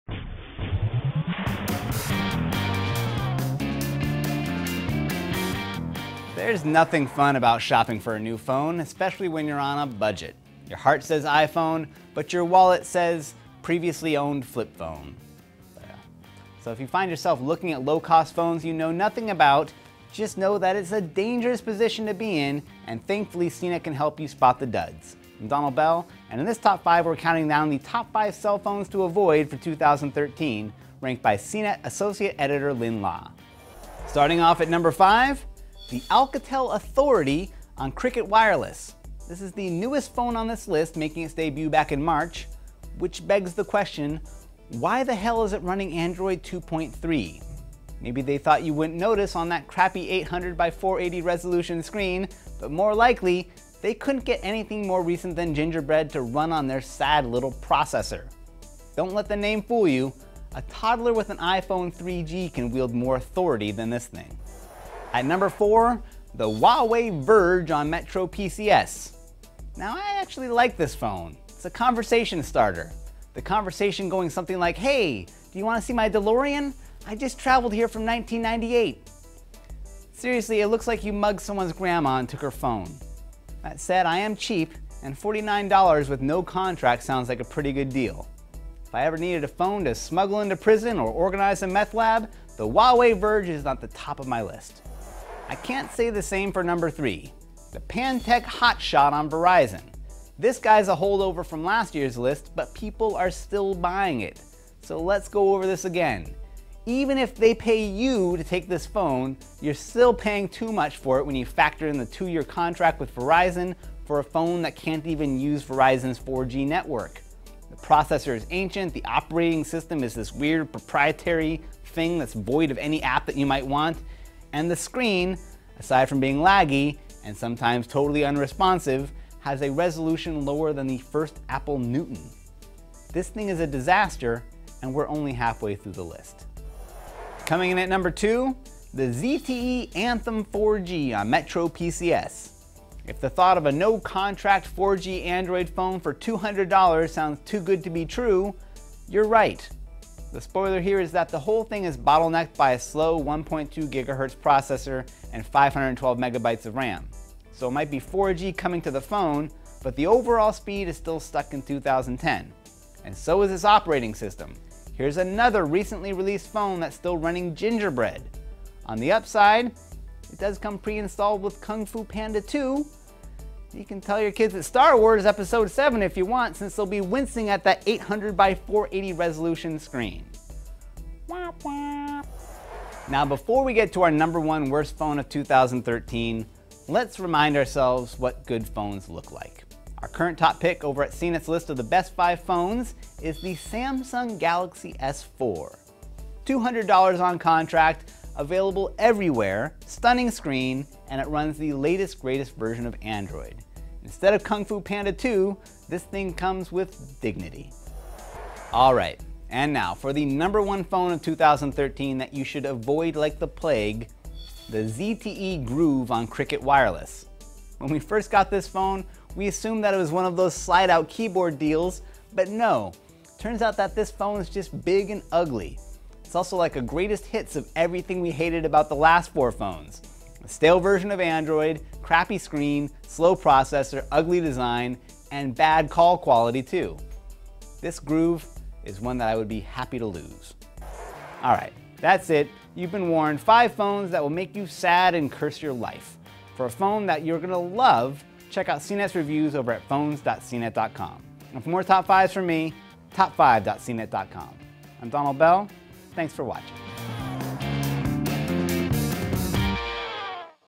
CNET Top 5 - Phones to avoid
There are some amazing, first-class cell phones out there. These aren't them.